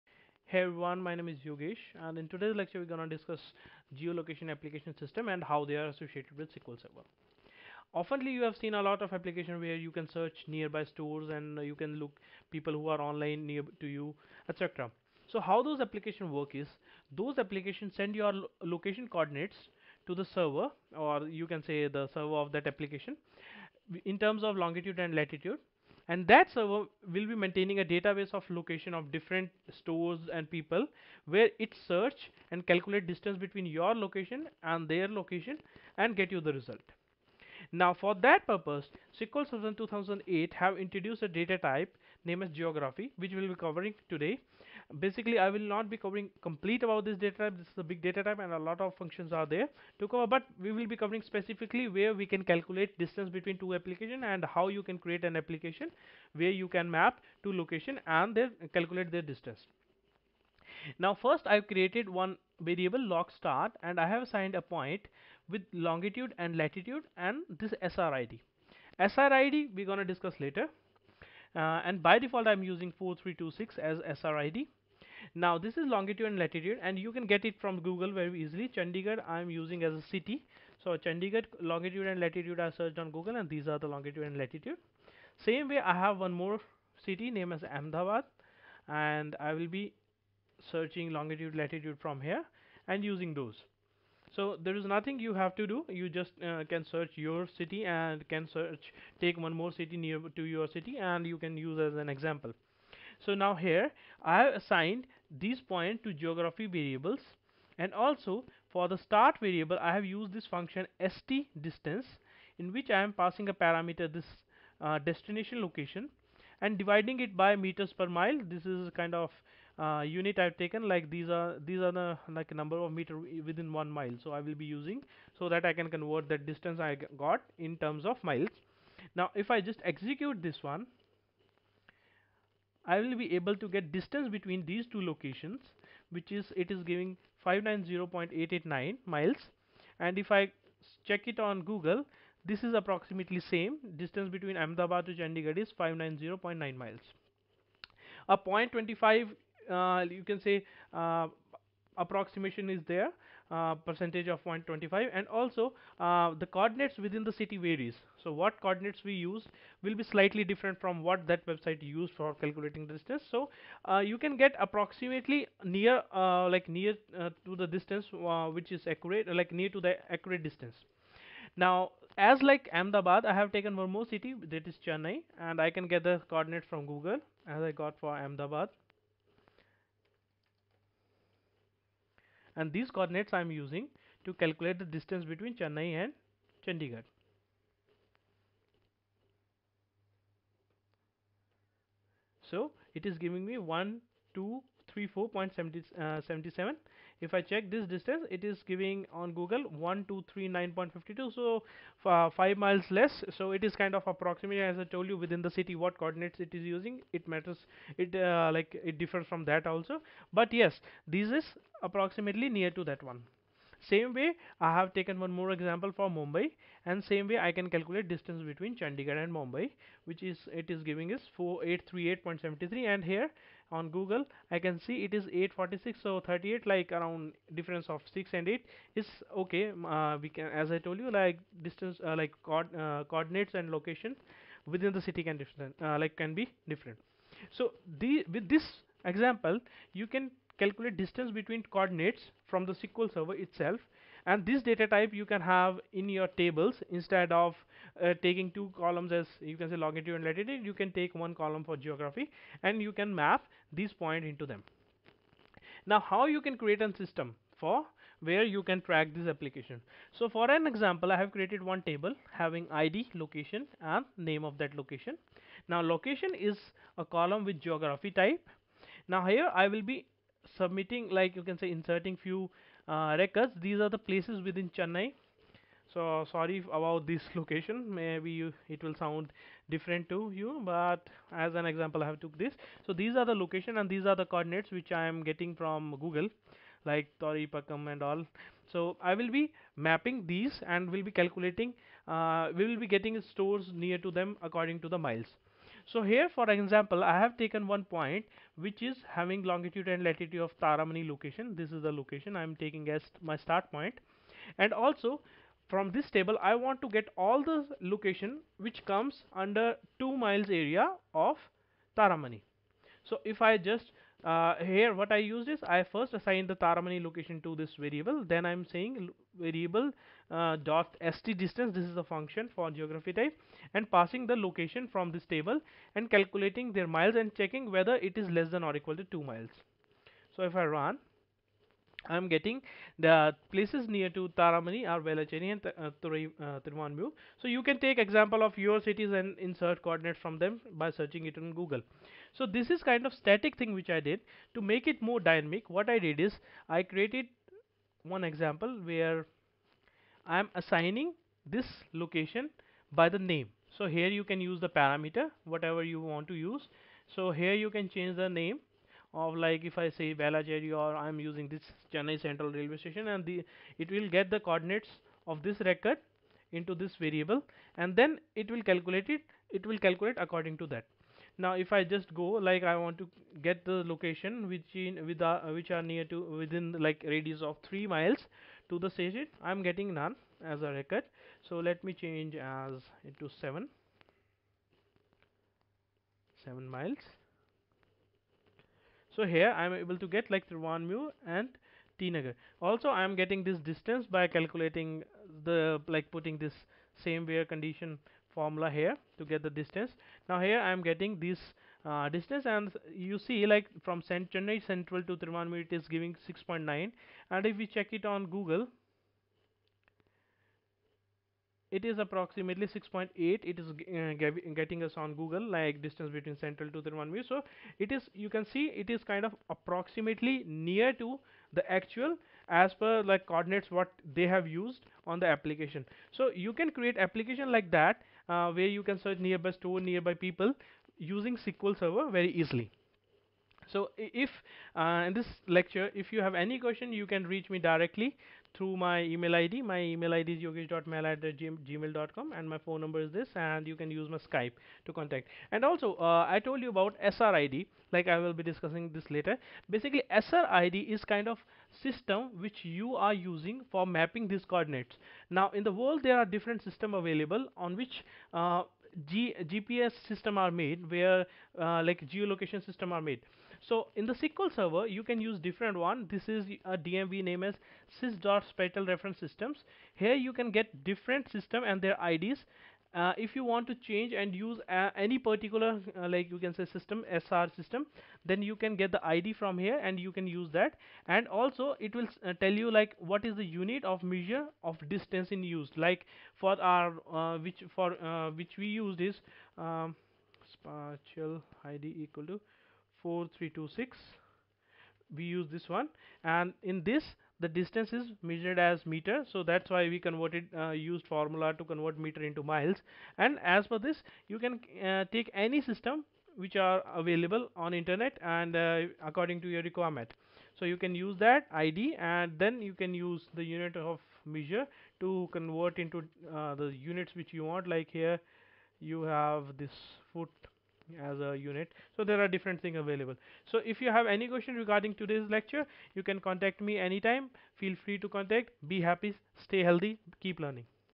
Calculating distance between two Points ( Longitude and Latitude) in SQL using GEOGRAPHY type
With geography datatype you can create system where you can filter locations on bases of distance between them. Also it make very easy to calculate distance between 2 location points. Along with that you can use any GPS standard system using SRID parameter in methods offered by Geography datatype.
For script check below link:

Building system where you can search locations near by N miles or kilometer